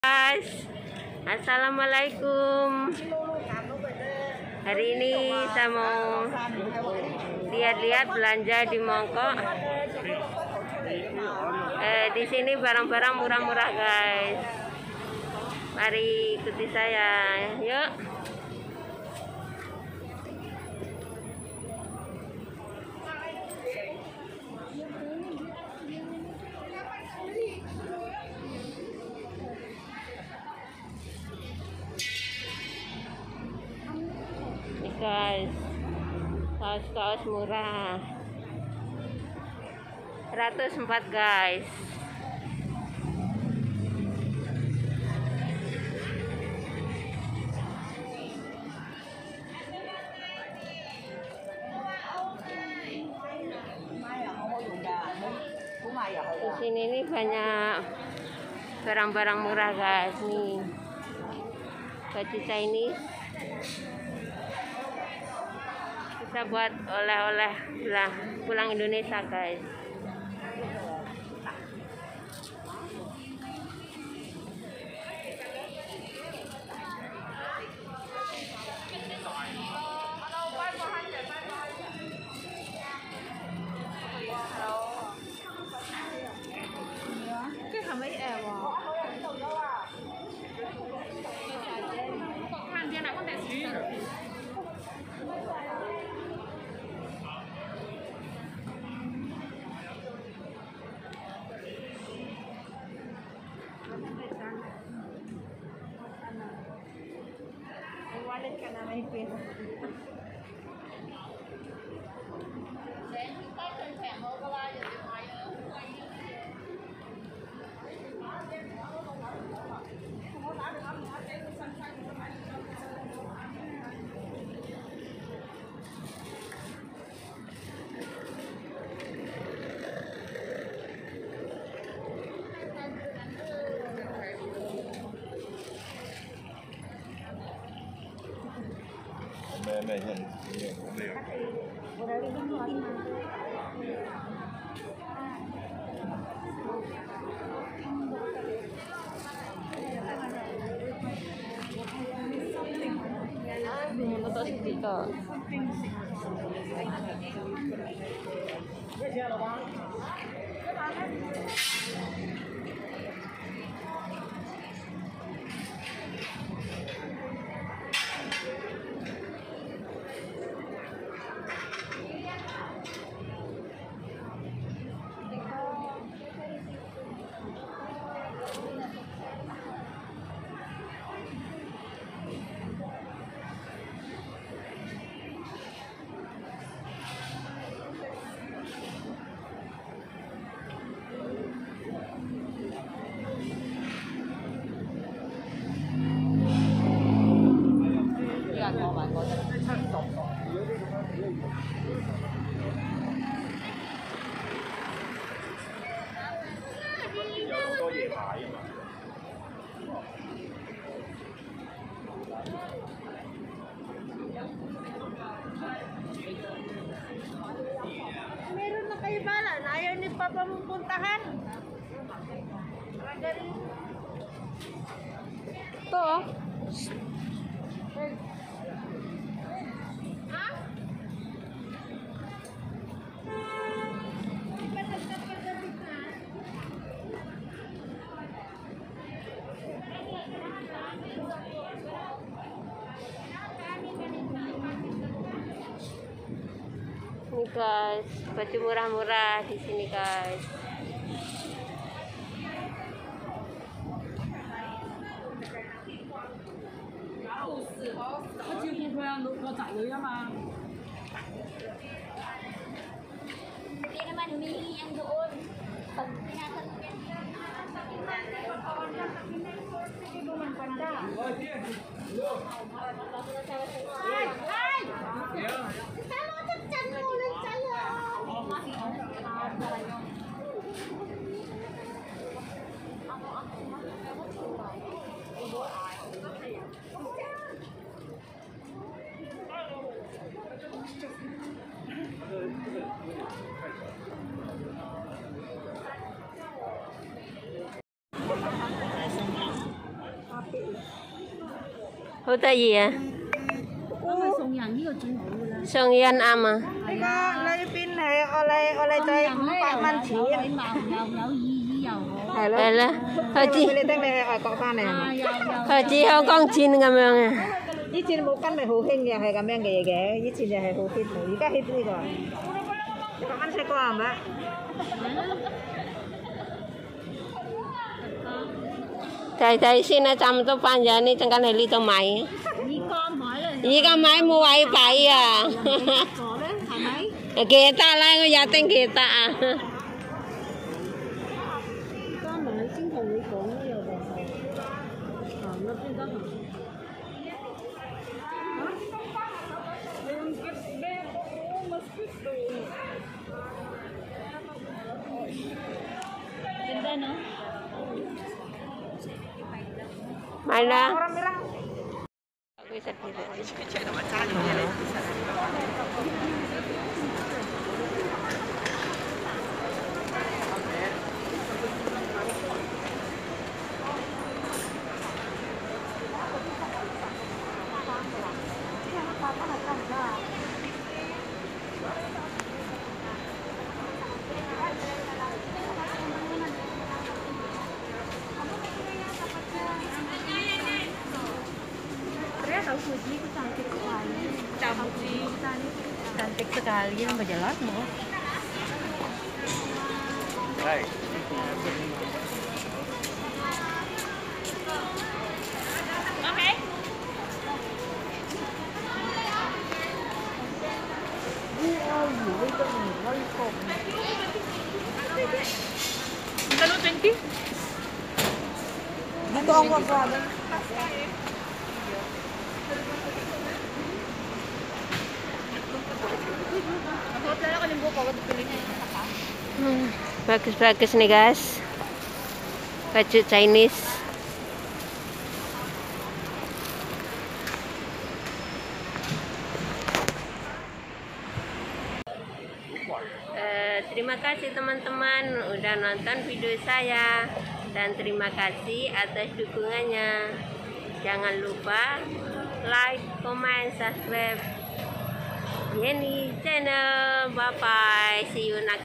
jalan jalan ke pasar mongkok Hongkong belanja dipasar turis murah
belanja dipasar murah Hongkong @yennichanel9459